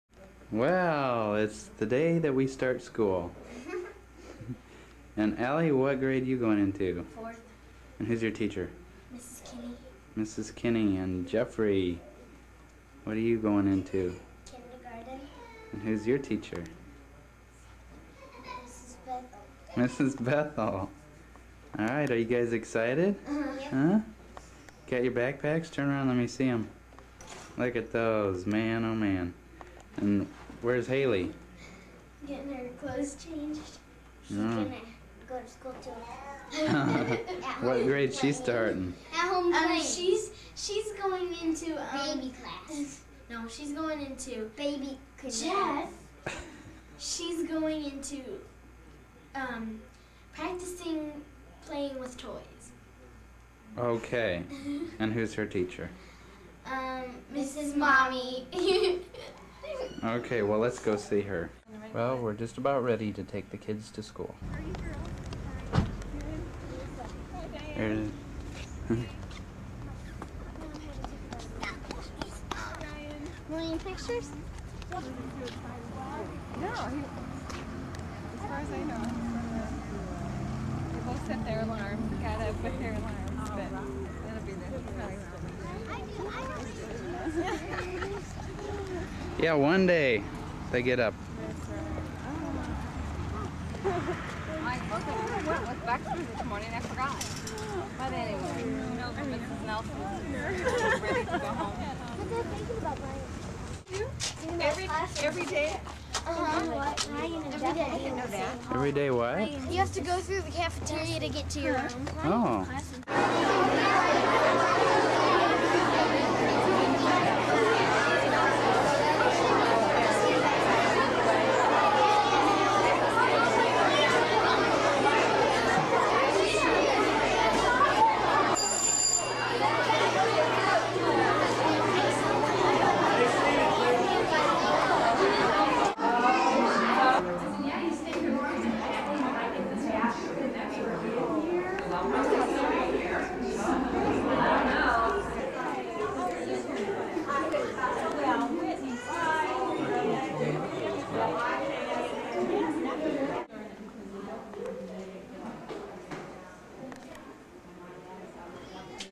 1st Day School 082592
Jeff's first day at school in August, 1992.  He just turned 21.